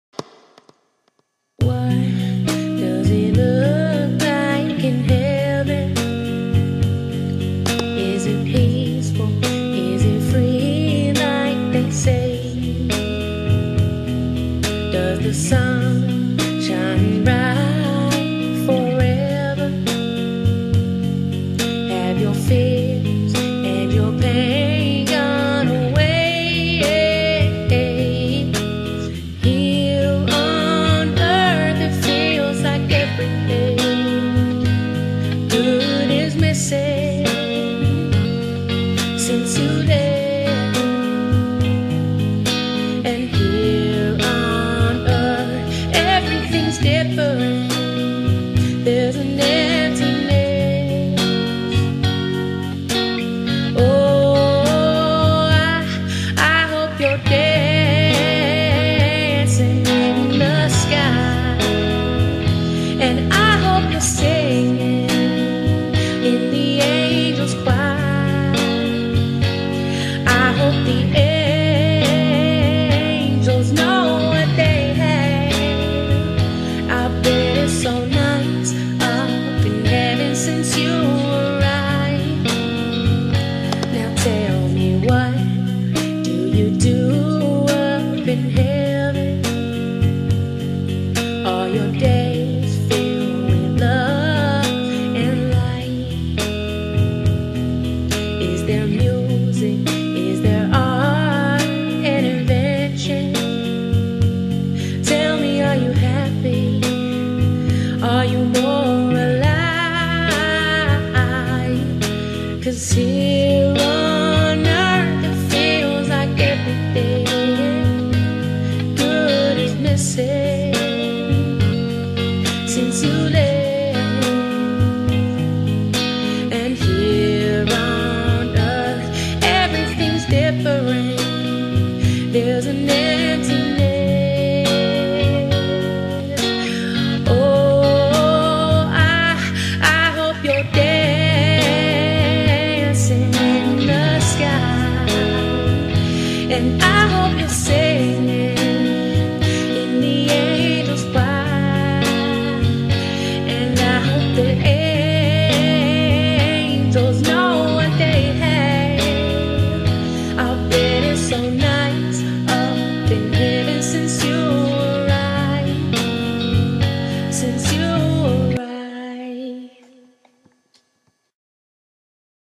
A memorial tribute to Claudette Joyce Chase
Claudette Joyce Chase, age 45, of Poplar, MT passed away Friday, January 3, 2014 at the Poplar Hospital.